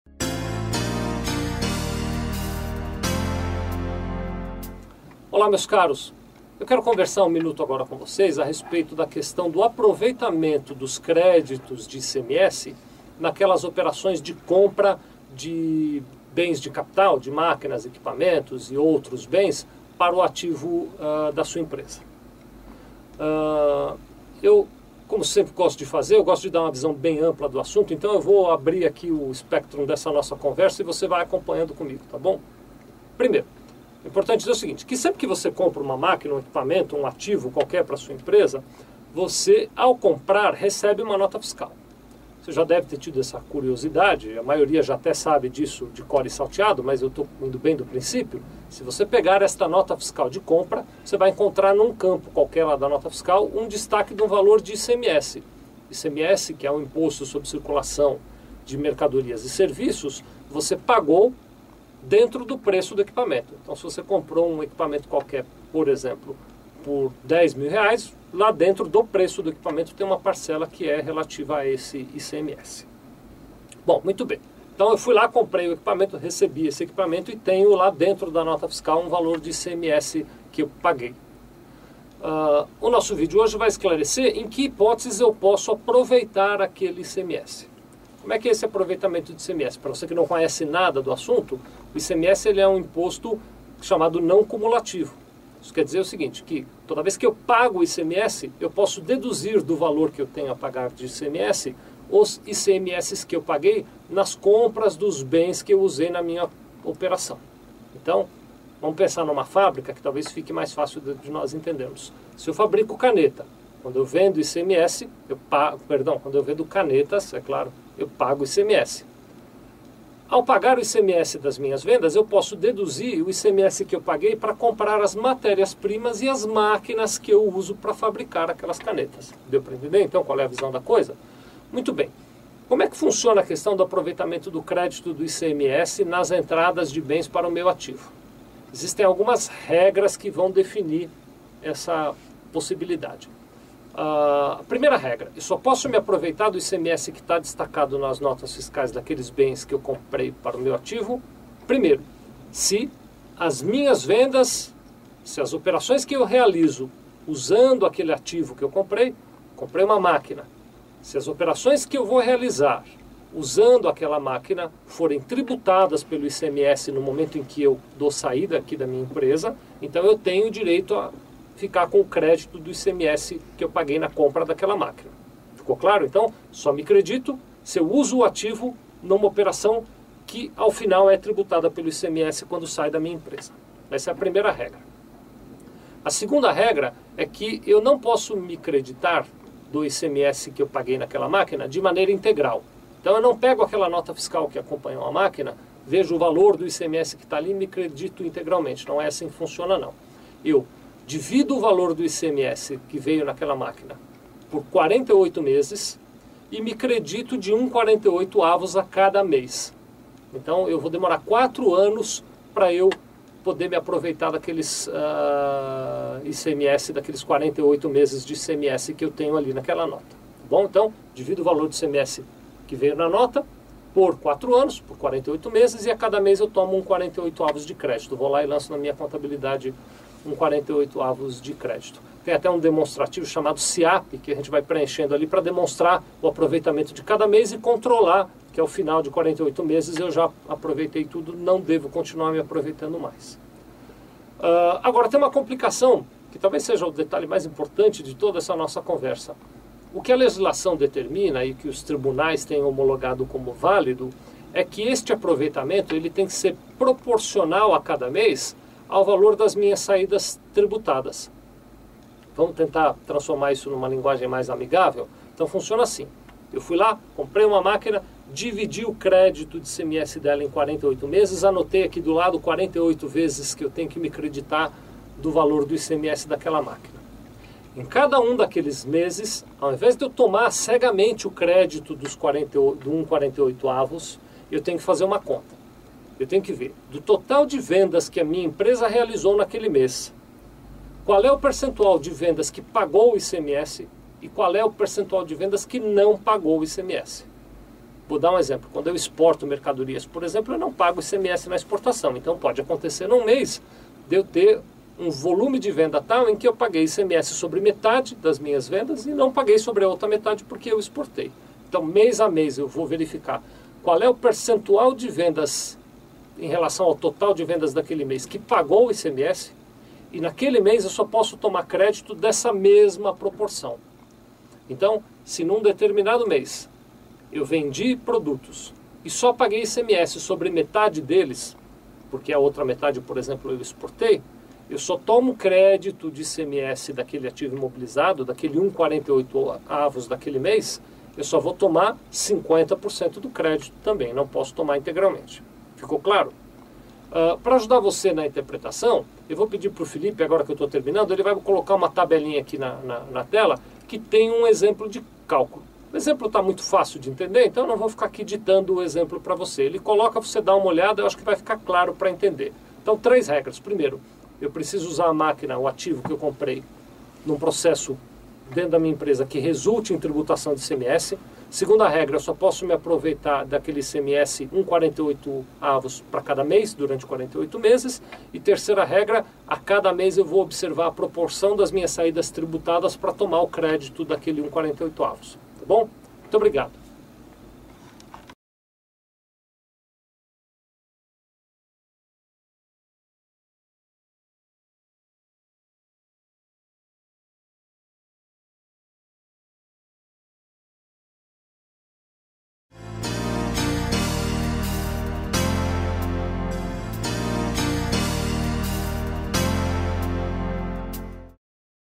ICMS Créditos de Entradas para o Ativo -- Sevilha Contabilidade
ICMS Créditos de Entradas para o Ativo -- Sevilha Contabilidade -- Vicente Sevilha Jr (tabela que está na lâmina Video Créditos de ICMS do arquivo anexo)
Sempre que uma empresa, contribuintes do ICMS, adquire bens para seu ativo imobilizado, que possuam vinculação com a cadeia produtiva do contribuinte, pode aproveitar os créditos referentes ao ICMS pagos no momento da compra. Porém há regras que precisam ser observadas.